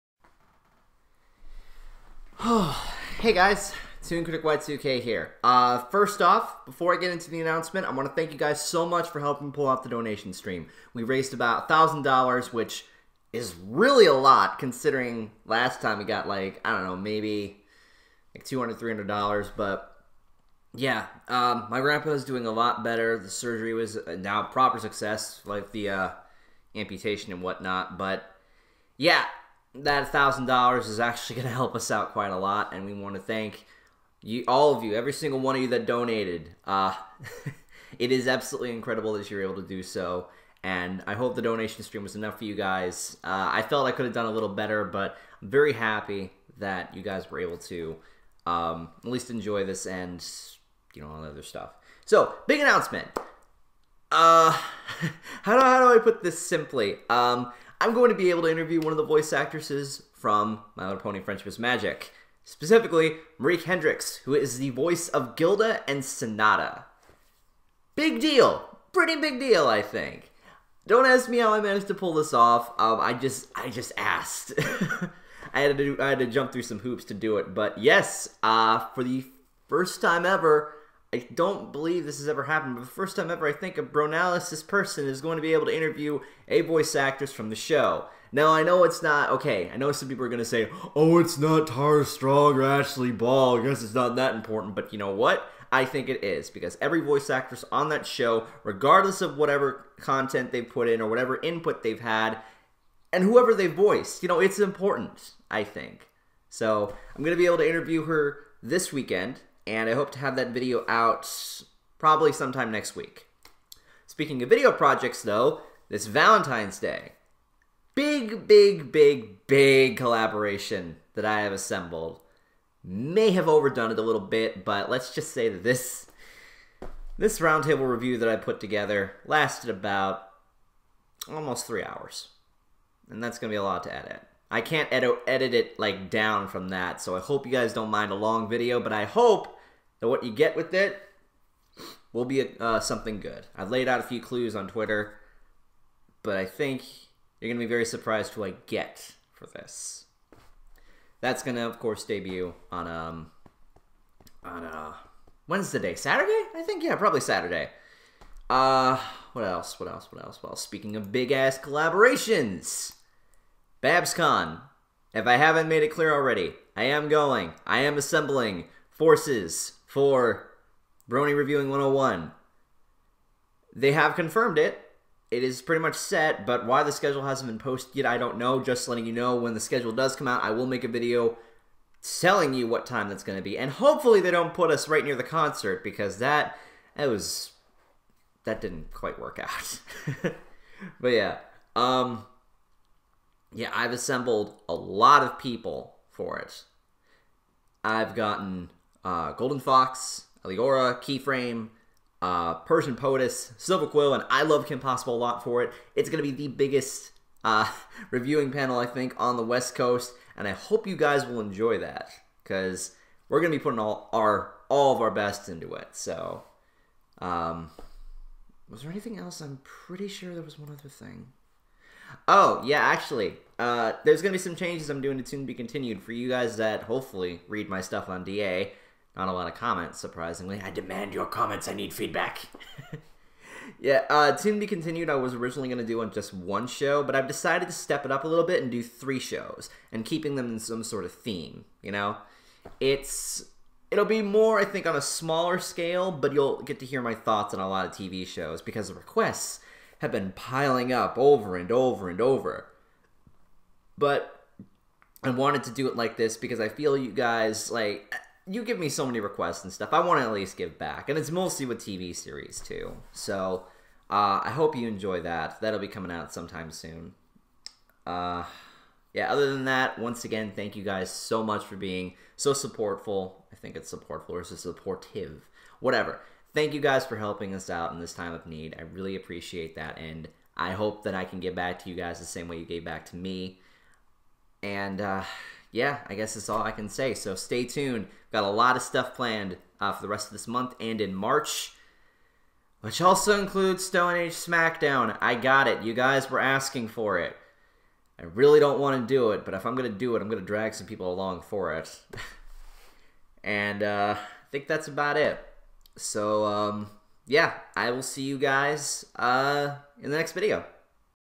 YOU'RE GONNA INTERVIEW GILDA?! (Also Updates!) - ToonKriticY2K
Other ToonKriticY2K re-uploads Original Upload: February 12, 2015 on ToonKritic2YK's deleted youtube channel

Original Description below:
And yes, it's all true. :3 Also, updates!